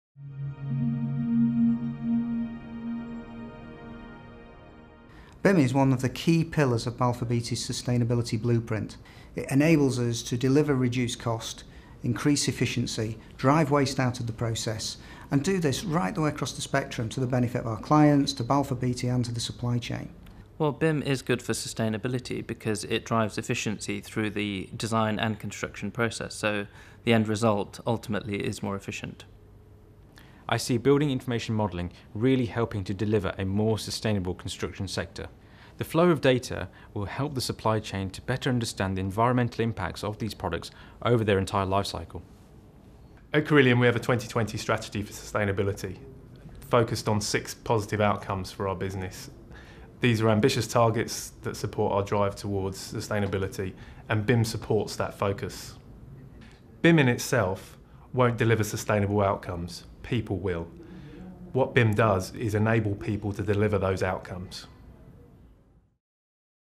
BIM and sustainability
Several of our partner companies discuss the links between using BIM and building more sustainable projects.